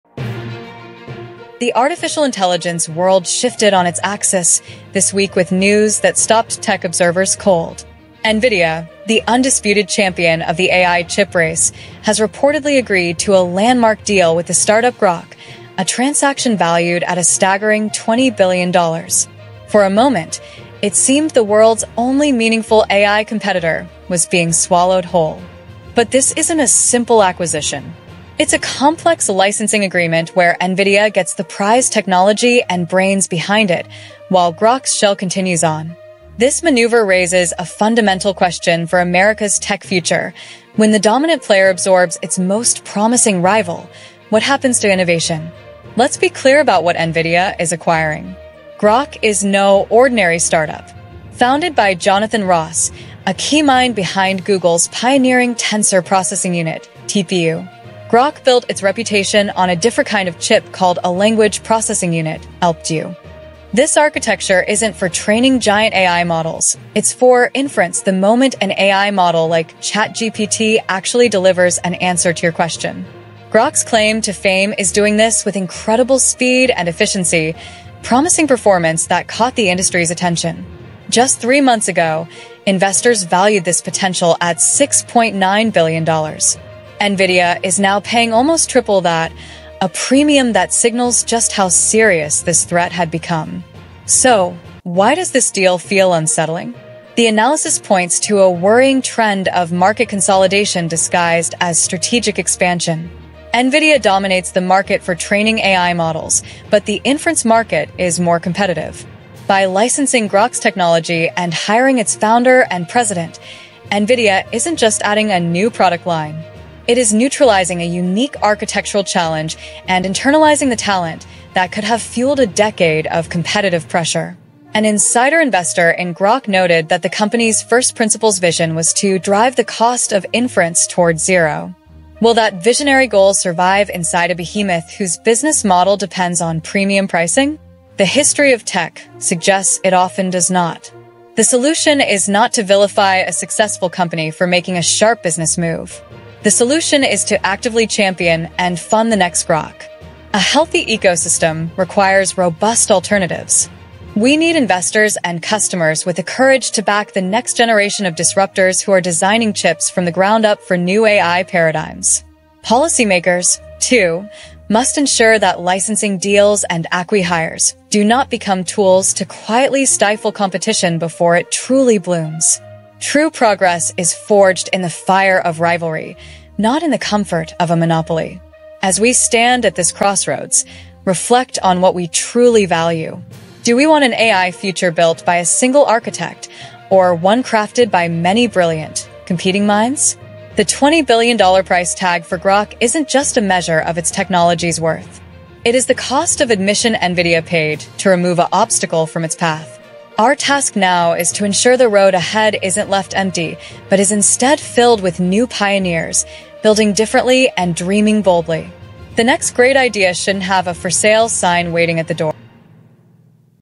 Groq Acquisition Explained: Did Nvidia Just Spend $20 Billion to Eliminate Competition?
The AI industry may have just crossed a historic turning point. Reports suggest Nvidia has agreed to a massive $20 billion Groq acquisition deal—but not in the traditional sense. Rather than a clean buyout, this move appears to be a complex licensing and talent acquisition arrangement that allows Nvidia to absorb Groq’s technology and leadership while leaving the startup’s corporate shell intact. The implications are enormous.

Groq is not just another AI startup. Founded by Jonathan Ross, one of the key architects behind Google’s Tensor Processing Unit (TPU), Groq developed a radically different approach to AI chips called the Language Processing Unit (LPU). Unlike GPUs, which excel at training massive AI models, Groq’s LPUs are optimized for inference—the moment when AI systems like ChatGPT generate real-time answers. Speed, efficiency, and lower cost are Groq’s defining advantages.

Just three months ago, Groq was valued at $6.9 billion. Nvidia’s willingness to pay nearly three times that valuation underscores how serious a competitive threat Groq had become. When people search for groq acquisition, they aren’t just asking about a deal—they’re questioning the future structure of the AI hardware market.

Nvidia already dominates AI model training. Inference, however, was one of the last frontiers where real competition existed. By licensing Groq’s architecture and bringing its founder and president in-house, Nvidia isn’t simply expanding its portfolio—it’s neutralizing an alternative vision for AI computing. One Groq investor famously said the company aimed to “drive the cost of inference toward zero.” Can that mission survive inside a company built on premium pricing?

This raises broader concerns about consolidation disguised as innovation. History shows that when dominant players internalize disruptive threats, long-term competition often suffers. The answer isn’t to condemn Nvidia for smart strategy—but to ask whether investors, customers, and policymakers are doing enough to support the next Groq.

The groq acquisition may define the AI landscape for years to come. The real question is whether the road ahead will be shaped by one architect—or many competing minds pushing each other forward.